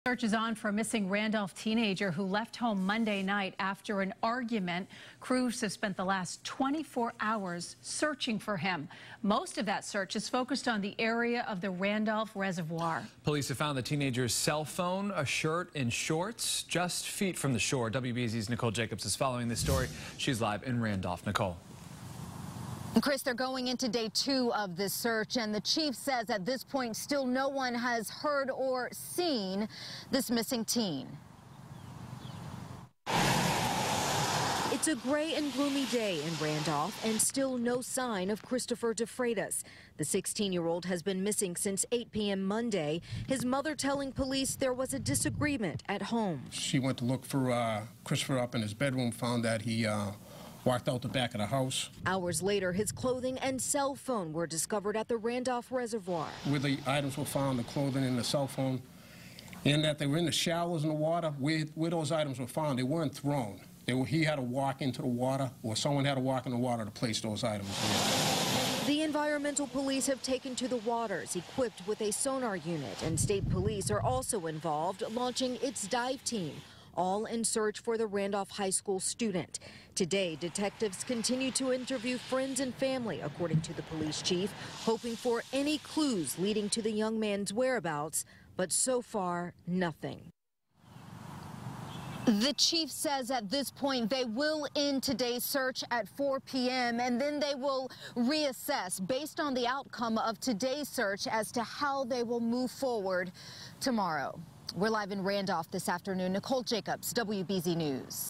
Search For Missing Randolph Teen Enters Second Day
His phone, shirt and shorts were found near a reservoir.  WBZ-TV's Nicole Jacobs reports.